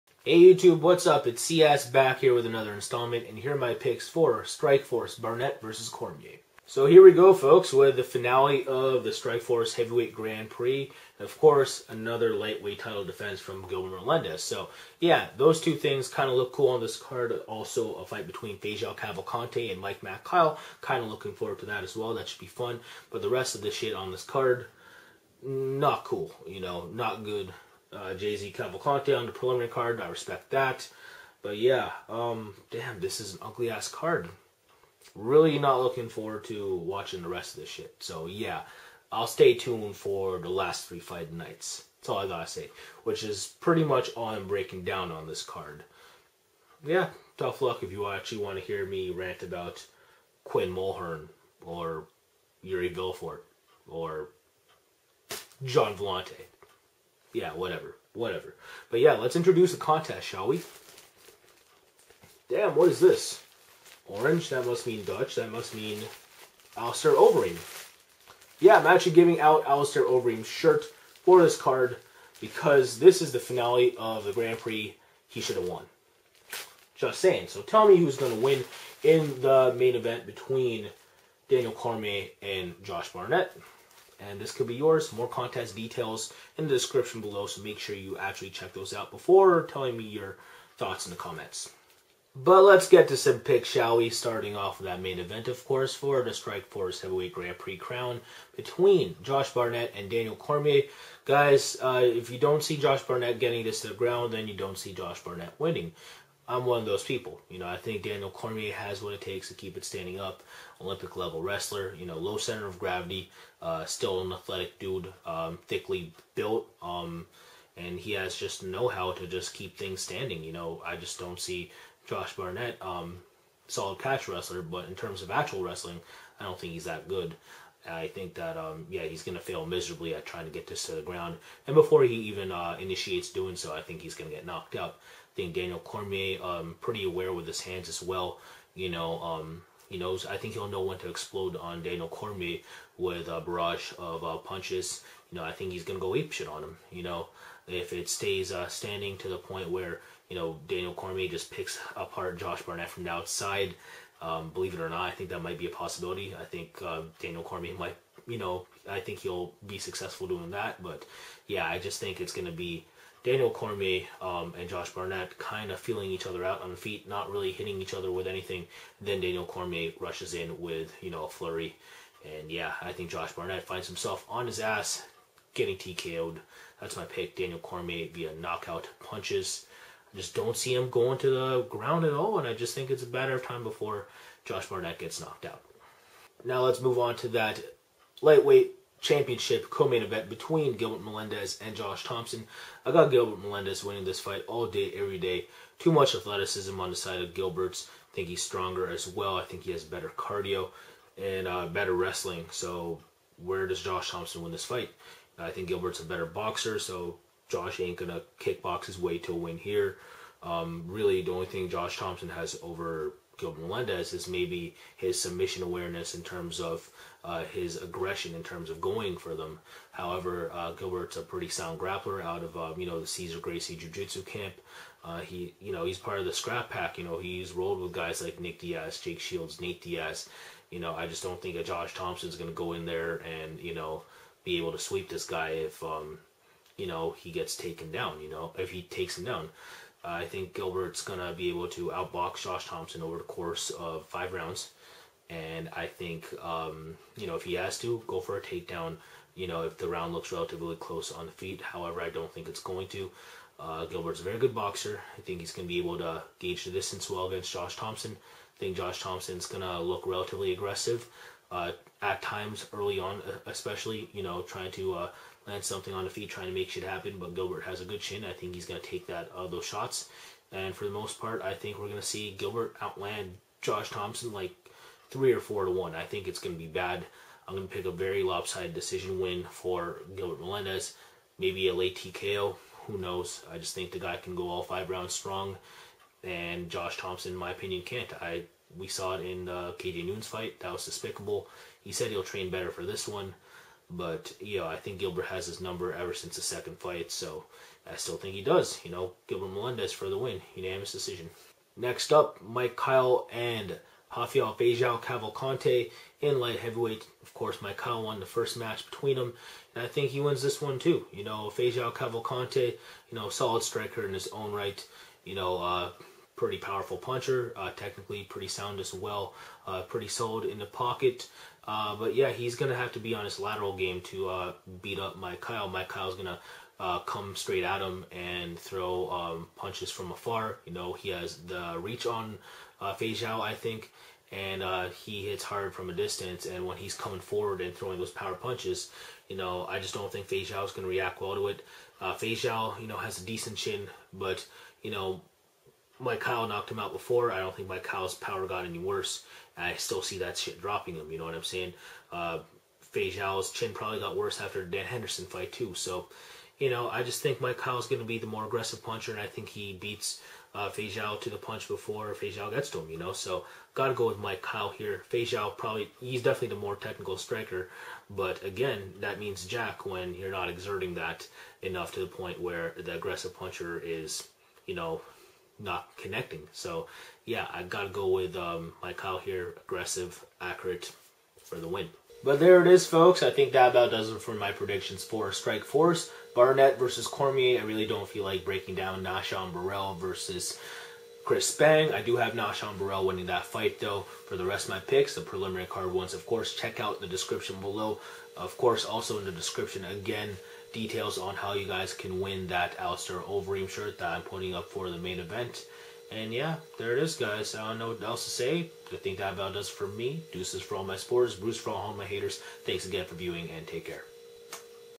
Strikeforce: Barnett vs. Cormier Predictions & Contest by CascadeSoldier
I don't want to see a grand prix from Strikeforce ever again. Actually, I don't want to see Strikeforce ever again. Thoughts on this card? Leave me them in the comment section below! Happy arguing!

Main card predictions:
Daniel Cormier via 1st Rd. KO (punches)
Gilbert Melendez via Unanimous Decision
Mike Kyle via 1st Rd. TKO (punches)
Nah-Shon Burrell via Unanimous Decision

Preliminary card predictions:
Gesias Cavalcante via Unanimous Decision
Virgil Zwicker via 1st Rd. TKO (punches)
Gianpiero Villante via 1st Rd. TKO (punches)
Quinn Mulhern via Unanimous Decision
Bobby Green via Unanimous Decision

Contest details:
- Leave a comment or video response telling me who will win the main event between Josh Barnett and Daniel Cormier, along with the method and round if you are picking a finish
- Picks can be left in the comment section below before the event's first fight, all others left any time after that will be voided
- The one winner will be picked at random if numerous subscribers have picked the same result that actually occurred for the contest fight
- If the fight is scrapped for whatever reason, the prize will be given out to a correct main event prediction with the previous points' criteria

Click on the link below for a better look at the contest prize [Size L only]: